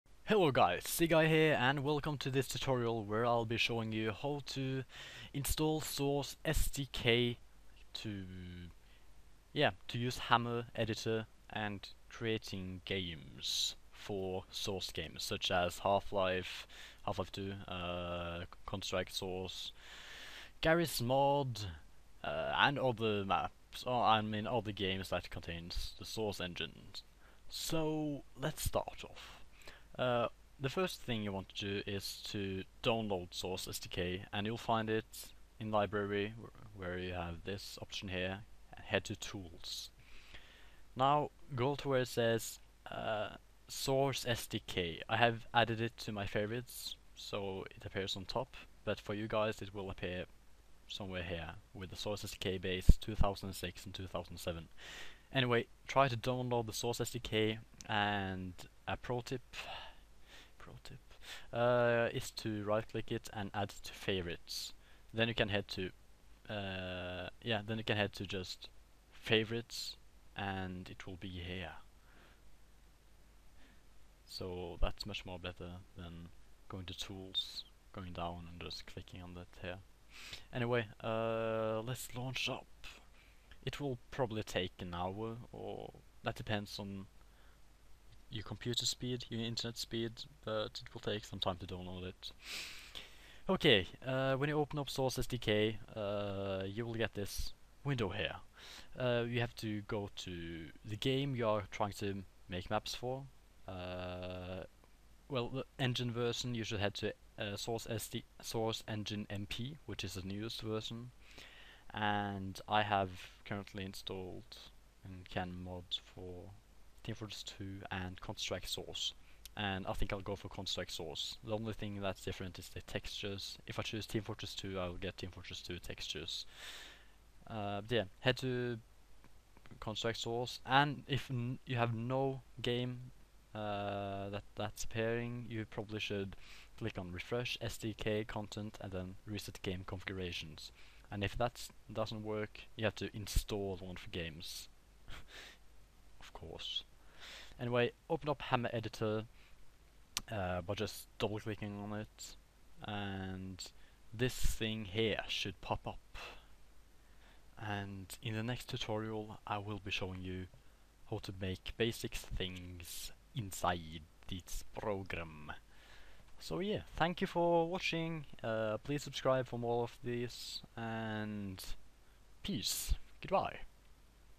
How to Download and Set Up Source SDK
In this tutorial, I will show you how to download and set up Source SDK.